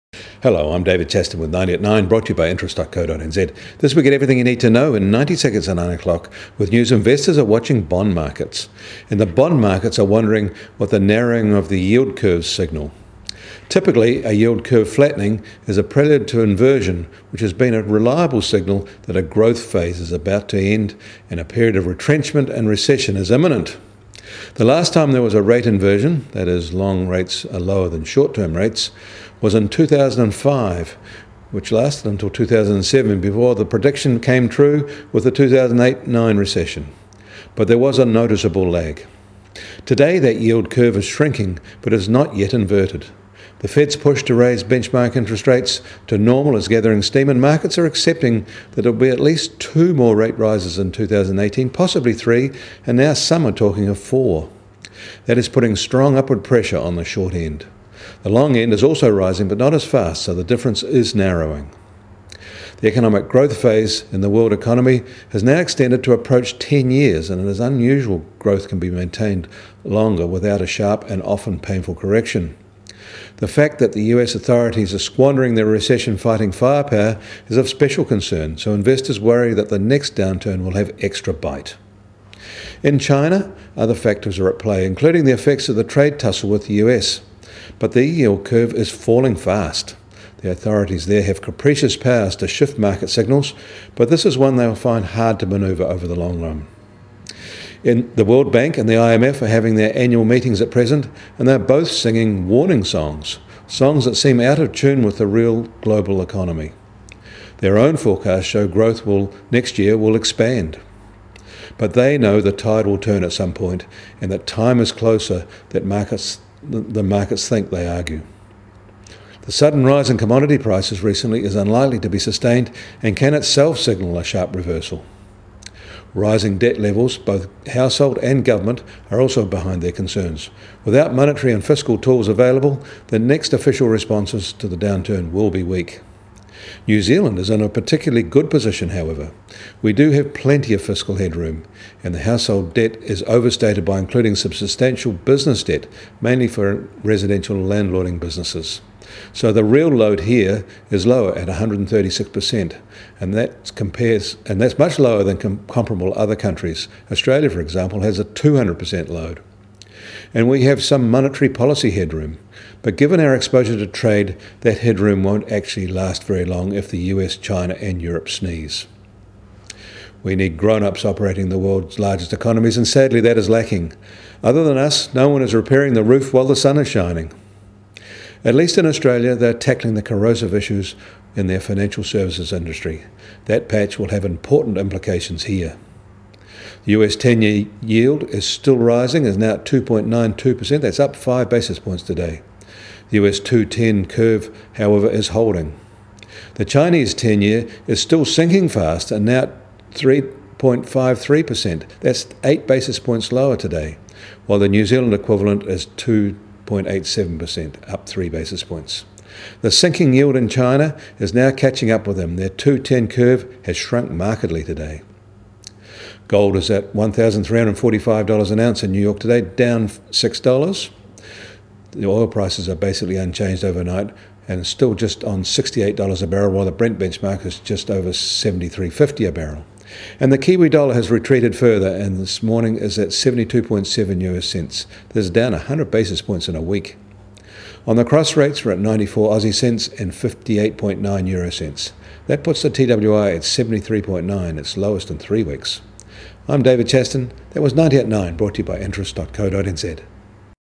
90 seconds @ 9am : Eyes on yield curves
Investors are watching bond markets.
And the bond markets are wondering what the narrowing of the yield curves signal.